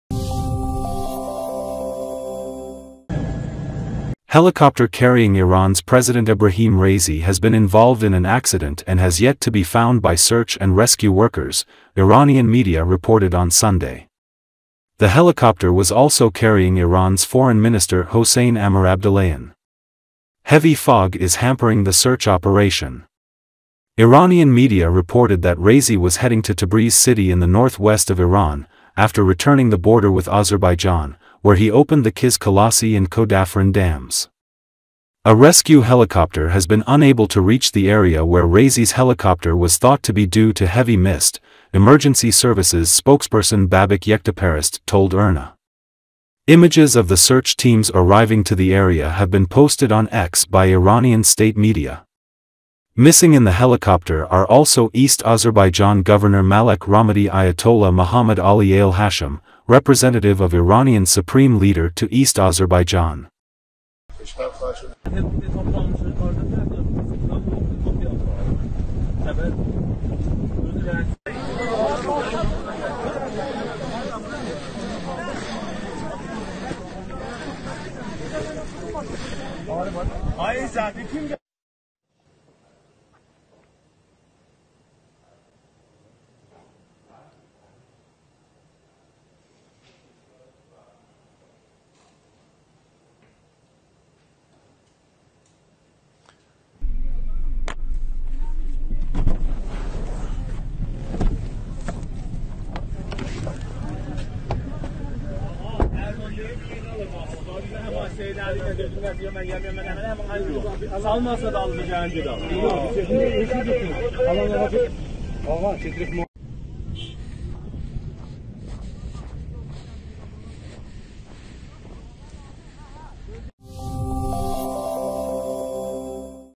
Helicopter carrying Iran’s President crashed: President and other officials are missing- State media
Helicopter carrying Iran’s President Ebrahim Raisi has been involved in an accident and has yet to be found by search and rescue workers, Iranian media reported on Sunday. The helicopter was also carrying Iran's Foreign Minister Hossein Amirabdollahian. Heavy fog is hampering the search operation. Iranian media reported that Raisi was heading to Tabriz city in the north west of Iran, after returning the border with Azerbaijan, where he opened the Qiz Qalasi and Khodaafarin dams. A rescue helicopter has been unable to reach the area where Raisi’s helicopter was thought to be due to heavy mist, emergency services spokesperson Babak Yektaparast told IRNA. Images of the search teams arriving to the area have been posted on X by Iranian state media. Missing in the helicopter are also East Azerbaijan Governor Malek Rahmati Ayatollah Mohammad Ali Ale-Hashem, representative of Iranian Supreme Leader to East Azerbaijan.